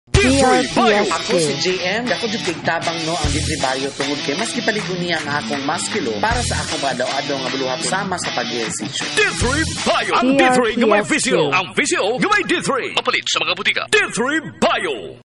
D3 BIO Testimonial Radio Ad 2020-2022 15s (in Cebuano, Version 14)
This is the fourteenth revision of the Clinica D3 BIO Testimonial Radio Ad, based in Cebuano Language.

DRPS Two 2563: D3 BIO Testimonial Radio Ad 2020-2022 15s (in Cebuano, Version 14)